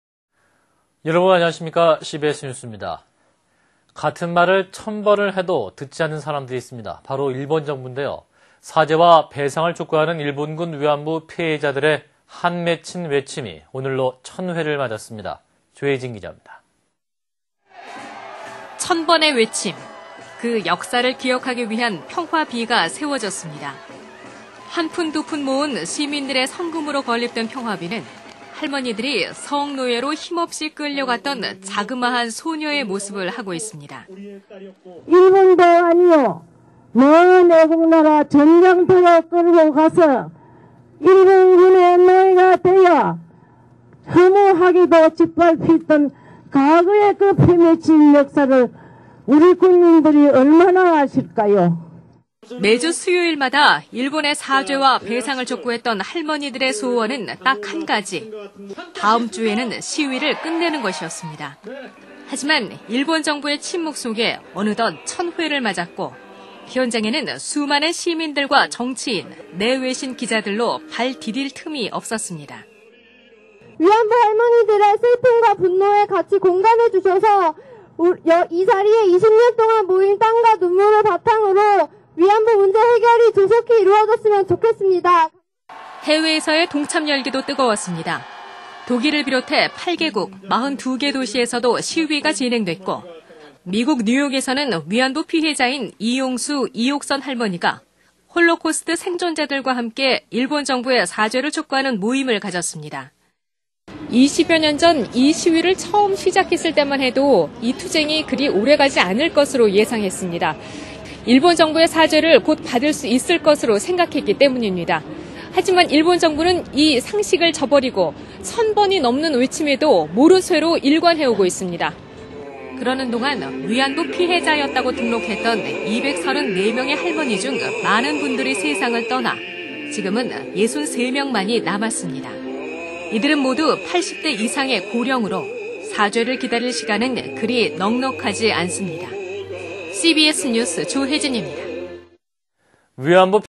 정대협 수요집회 1천회..이룬 것과 남긴 것은?
[ 앵커 멘트 ]
같은 말을 천 번을 해도 듣지 않는 사람들이 있습니다.
바로 일본정부인데요.
사죄와 배상을 촉구하는 일본군위안부 피해자들의 한맺힌 외침이 오늘(어제)로 1천회를 맞았습니다.
[ 조혜진기잡니다. ]
천 번의 외침.
그 역사를 기억하기 위한 평화비가 세워졌습니다.
한푼 두푼 모은 시민들의 성금으로 건립된 평화비는 할머니들이 성노예로 힘없이 끌려갔던 자그마한 소녀의 모습을 하고 있습니다.
(김복동 할머니/일본군 위안부 피해자)
"일본도 아닌, 먼 외국나라 전쟁터로 끌고가서 일본군의 노예가 되어 허무하게도 짖밟혔던 과거의 그 피맺힌 역사를 우리 국민들이 얼마나 아실까요?"
매주 수요일마다 일본의 사죄와 배상을 촉구했던 할머니들의 소원은 딱 한 가지. 다음 주에는 시위를 끝내는 것이었습니다.
하지만, 일본정부의 침묵 속에 어느 덧 천회를 맞았고 현장에는 수많은 시민들과 정치인, 내외신 기자들로 발 디딜 틈이 없었습니다.
"위안부 할머니들의 슬픔과 분노에 같이 공감해 주셔서 이 자리에 20년 동안 모인 땀과 눈물을 바탕으로 위안부 문제 해결이 조속이 이뤄졌으면 좋겠습니다."
해외에서의 동참 열기도 뜨거웠습니다. 독일을 비롯해 8개국 42개 도시에서도 시위가 진행됐고, 미국 뉴욕에서는 위안부 피해자인 이용수, 이옥선 할머니가 홀로코스트 생존자들과 함께 일본정부의 사죄를 촉구하는 모임을 가졌습니다.
"20여년 전 처음 시위를 시작할 때만해도 이 투쟁이 그리 오래가지 않을 것으로 예상했습니다.
곧 일본의 사죄를 받을 수 있을 것이라 생각했기 때문입니다.
하지만 일본 정부는 이 상식을 저버리고 천 번의 외침에도 모르쇠로 일관하고 있습니다."
그러는 동안 위안부 피해자였다고 등록했던 234명의 할머니들 중 많은 분들이 세상을 떠나 63분만이 남았습니다.
이들은 모두 80대 이상의 고령으로 사죄를 기다릴 시간은 그리 넉넉하지 않습니다.